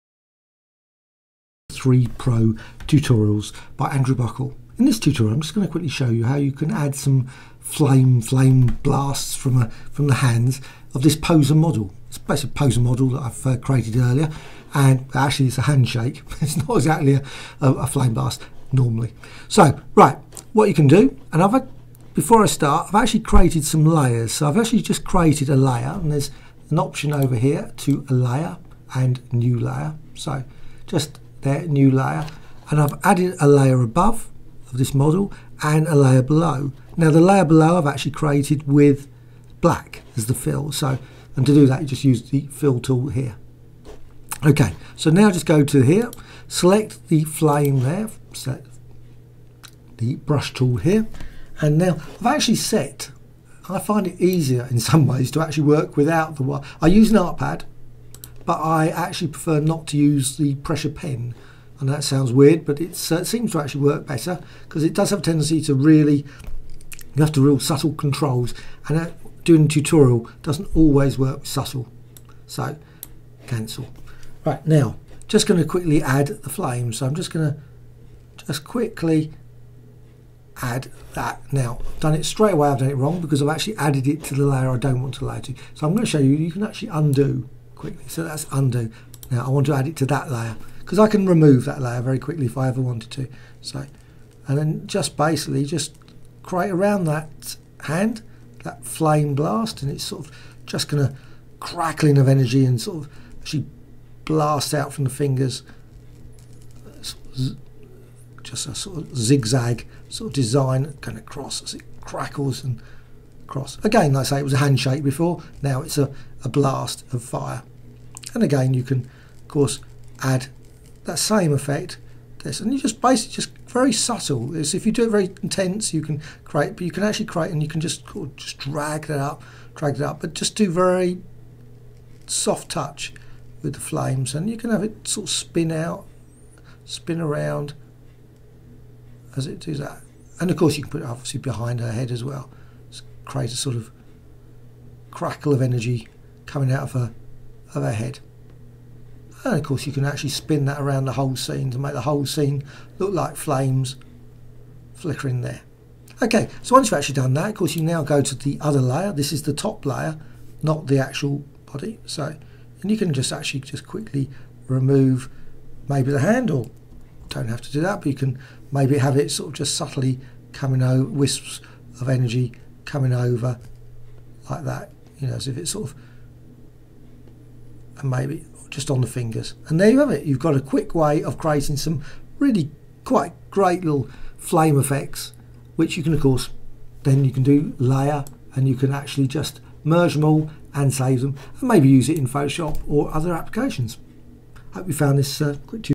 Flame Painter Pro : Adding super power flames to female character tutorial
Super powers in second ... The flame Painter  Pro application can be used to create flames across layers and you can use the tool to add super power flames to a female (or male) character - character created in Poser. Software tutorial - a very basic flame design added in Flame Painter